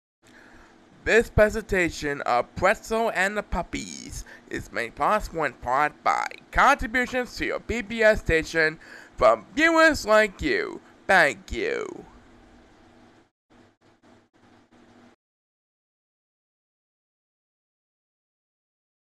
2022 Custom Funding Credits: Pretzel and the Puppies
This is a custom Viewers Like You style funding credits sequence for the Apple TV Plus animated series Pretzel and the Puppies.